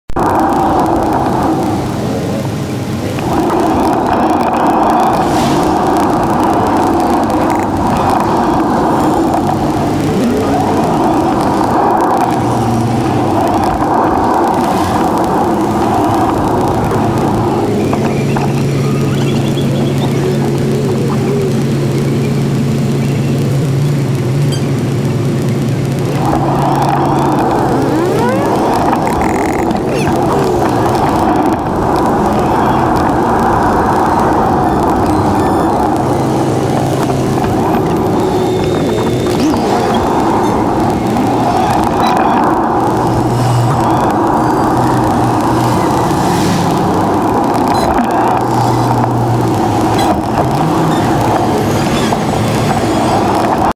Les bouchons çé la fôt aux cycliss et aux bus accordéons
Mes collègues autosolistes sont dans ces voitures. Ils arriveront 1h plus tard en râlant contre les bouchons qu'ils fabriquent.